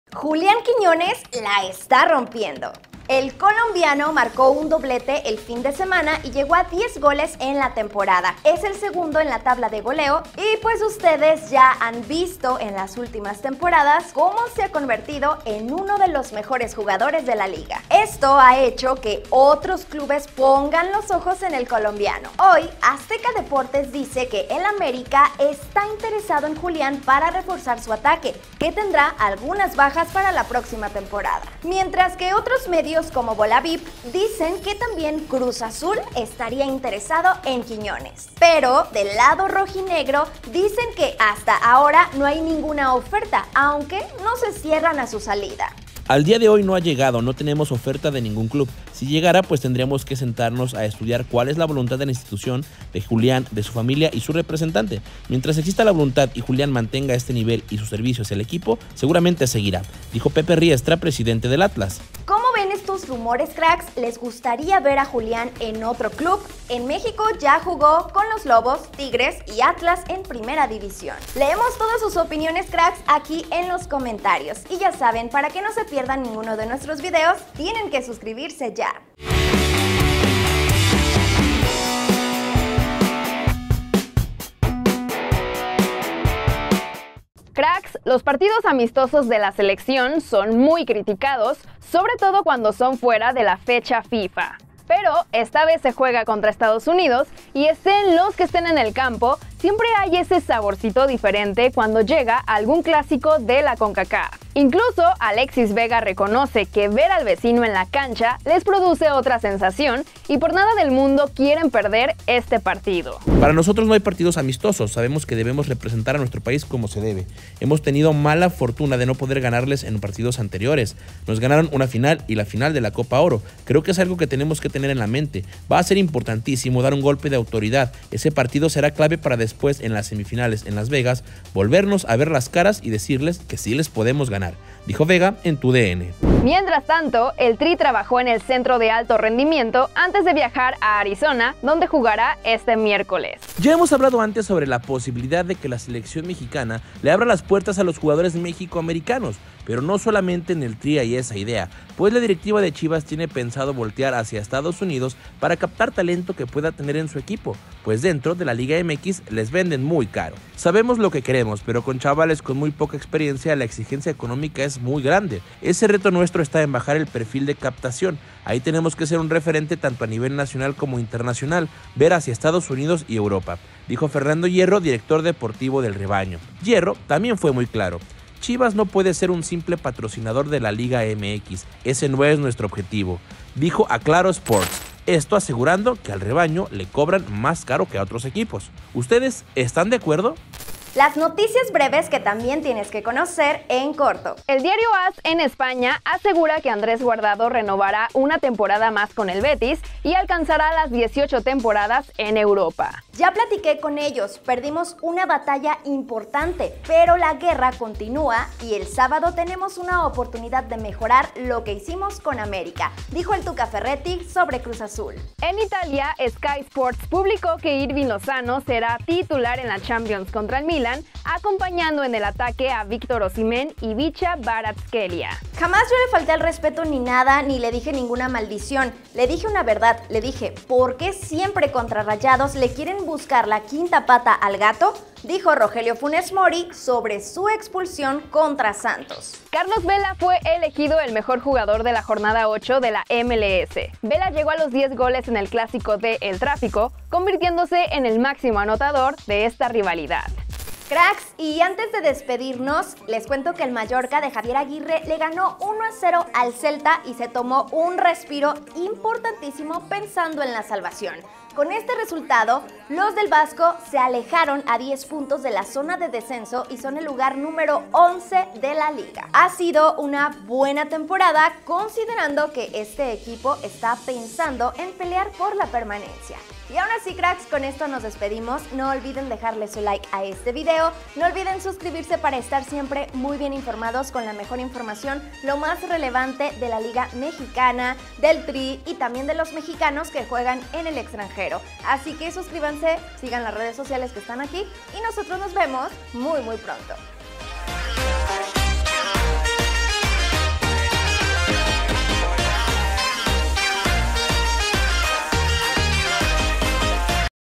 El AMÉRICA va por QUIÑONES | "Contra USA NO es AMISTOSO" | CHIVAS buscará MEXICOAMERICANOS
Julián Quiñones la está rompiendo y varios equipos grandes de la Liga Mx, lo tienen en la mira. Según varios rumores, América y Cruz Azul serían los clubes más interesados en el colombiano. Los partidos amistosos de la Selección son muy criticados, sobre todo cuando son fuera de la fecha FIFA. Pero esta vez se juega contra Estados Unidos y siempre hay un sabor diferente. Alexis Vega reconoce que ver al vecino en la cancha les produce otra sensación y por nada del mundo quieren perder este partido. En Chivas ya tienen pensado voltear a ver jugadores mexicoamericanos. Fernando Hierro fue quien dijo que Chivas no puede ser un simple patrocinador de la Liga Mx. Tuca Ferreti habló después de perder el Clásico Joven con América. Andrés Guardado renovará una temporada más con el Betis y alcanzará las 18 temporadas en Europa. En Italia, Sky Sports publicó que Hirving Lozano será titular en la Champions contra el Milan. Rogelio Funes Mori habló sobre su expulsión contra Santos. Carlos Vela fue elegido el mejor jugador de la Jornada 8 de la MLS. Vela llegó a 10 goles en el Clásico de El Tráfico, convirtiéndose en el máximo anotador de esta rivalidad.